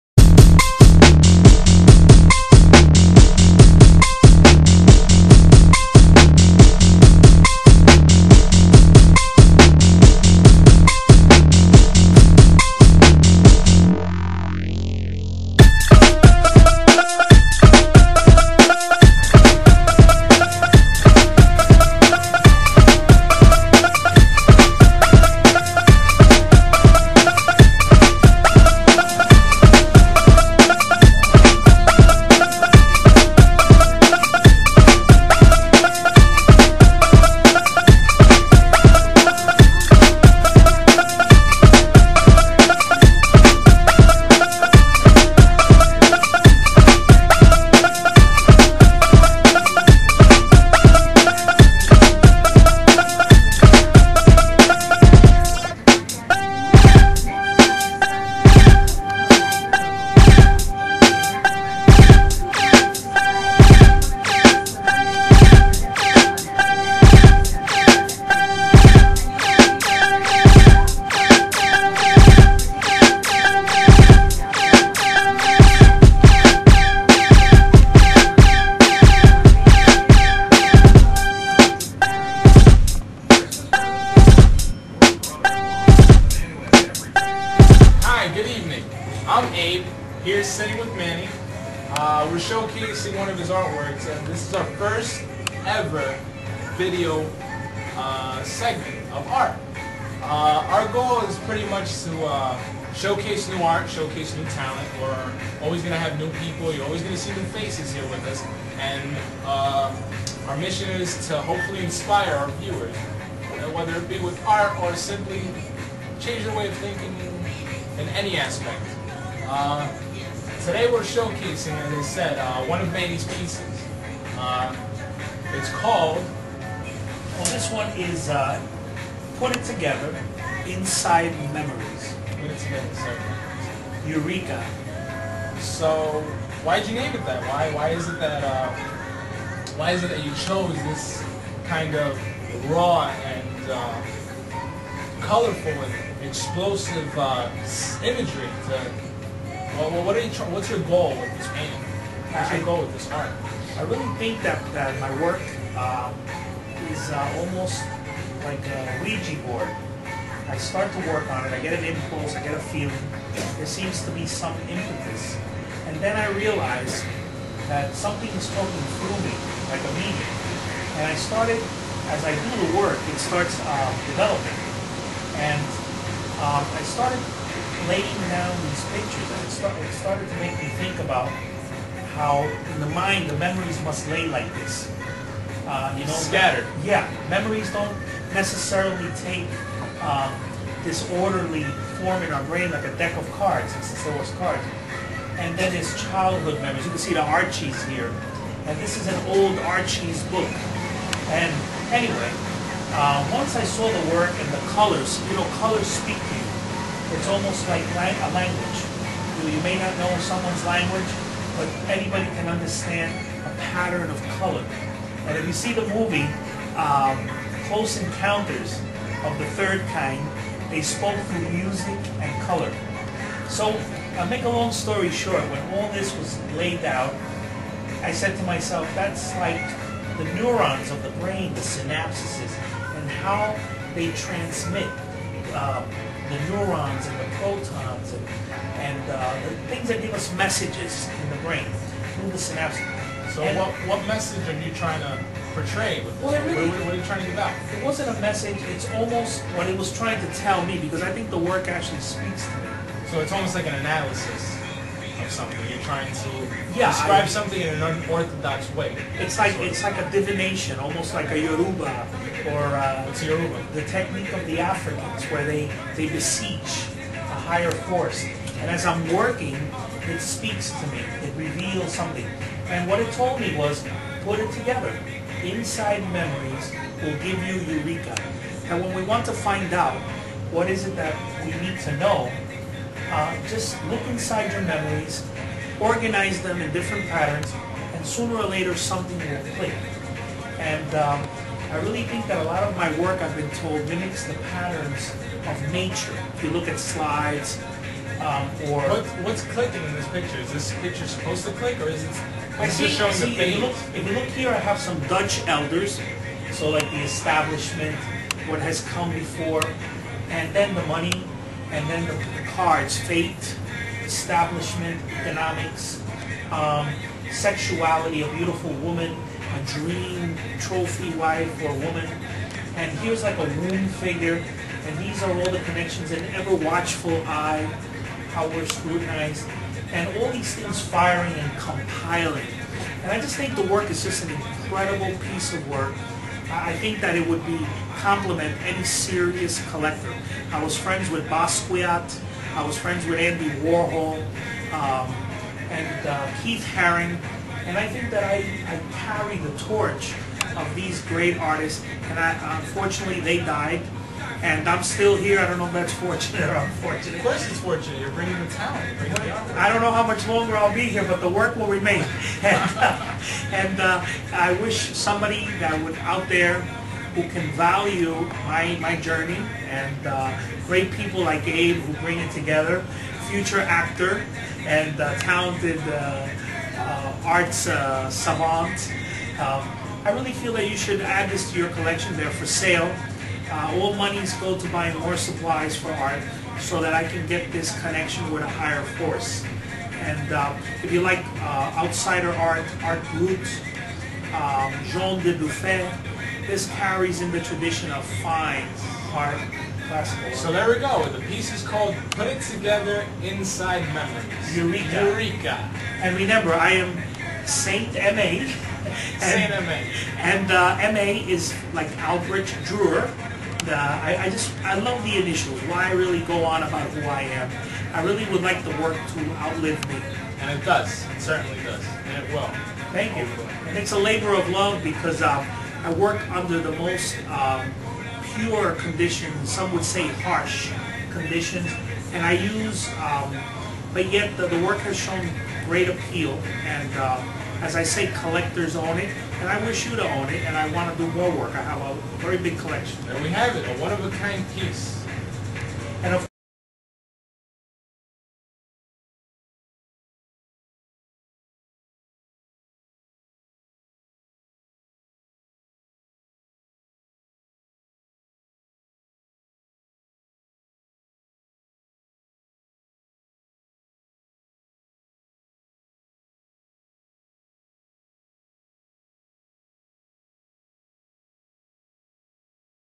Modern Art Sales Show 1
First Segment of the Modern Art Sales Show showcasing saint M.A.'s painting.